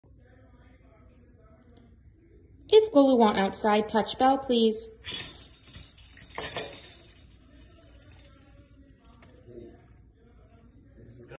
Touch bell🛎️ if want to go out 😃
Love touching bell, only then I get out of the house 🏡🏠 😃

I know how to get out side 😄 only touch bell and humans let me go out 🏠 😃